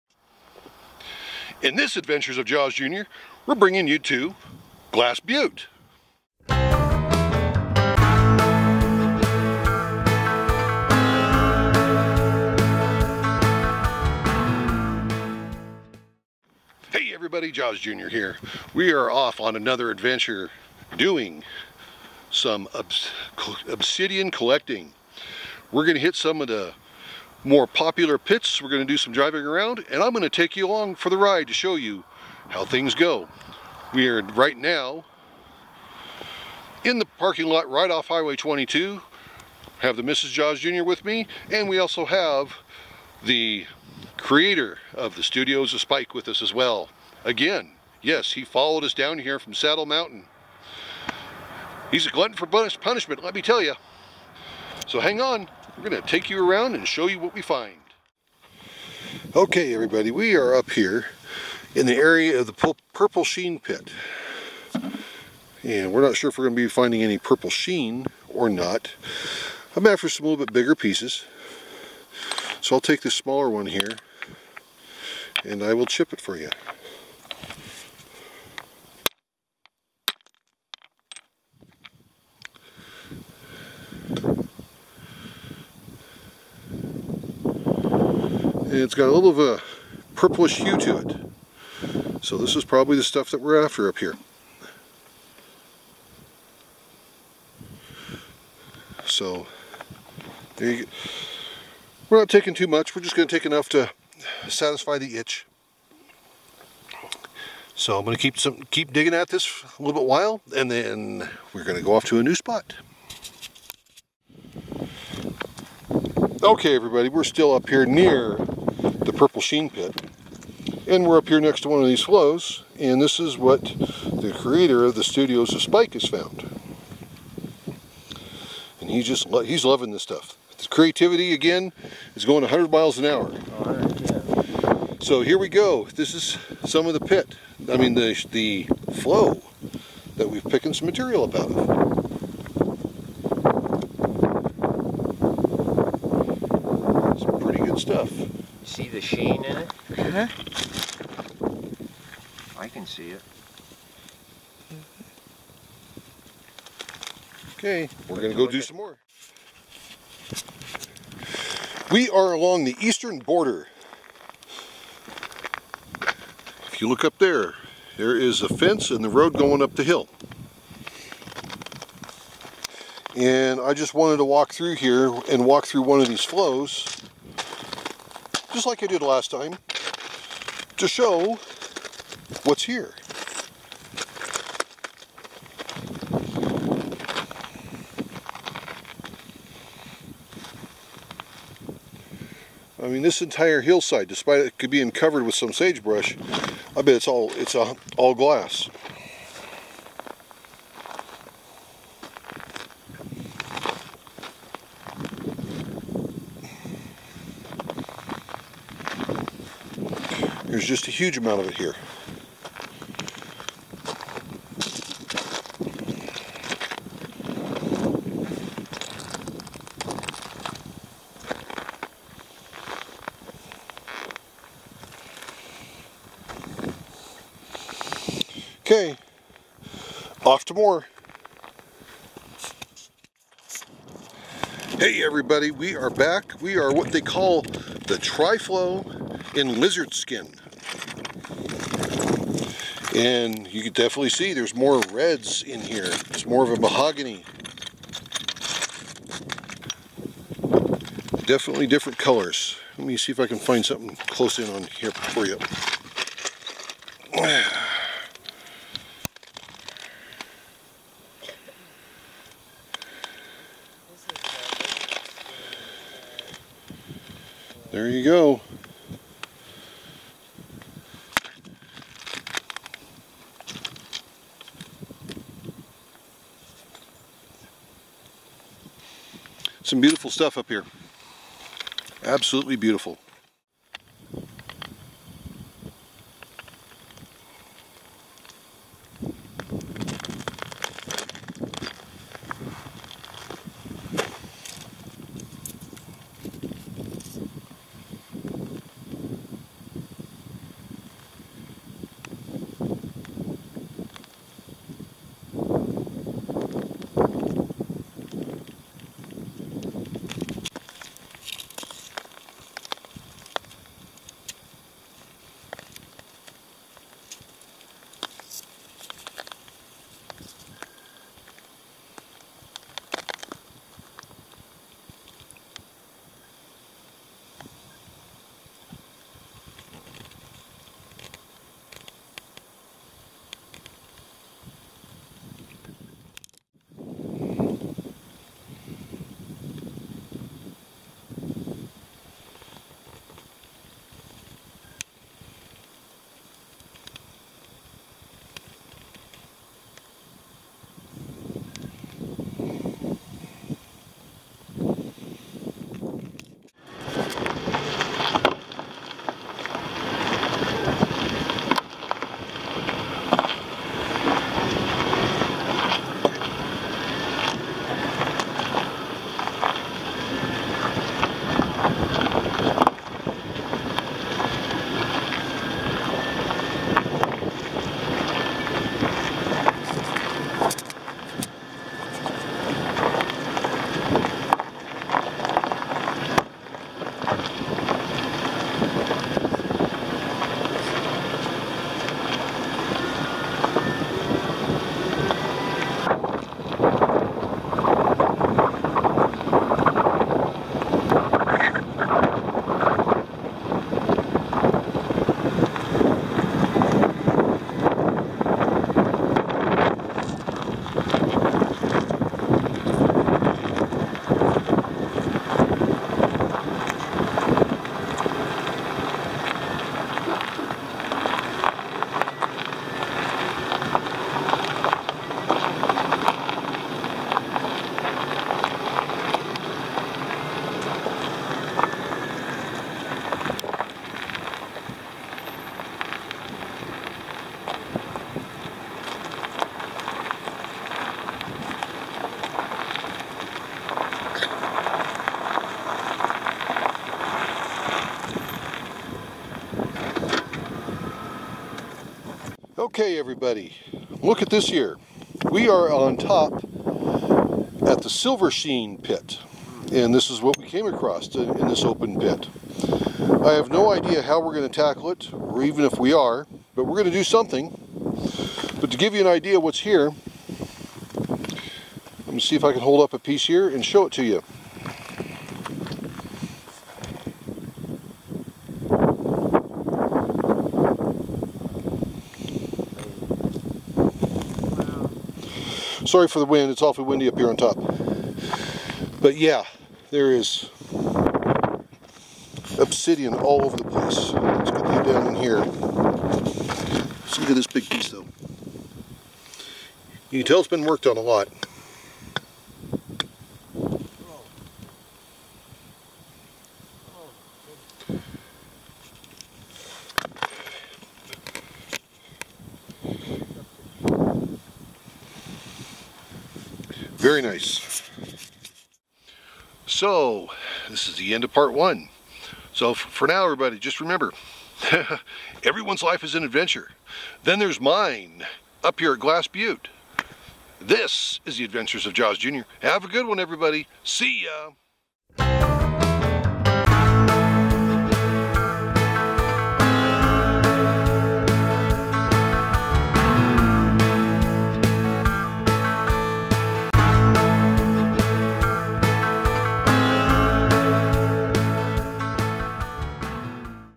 Glass Butte Part 1
In this Adventure, Jaws Jr takes us all to Glass Butte Oregon where we go through a few different dig sites and find some beautiful obsidian. This is the first of two videos!  This is a place for everyone to go and collect some!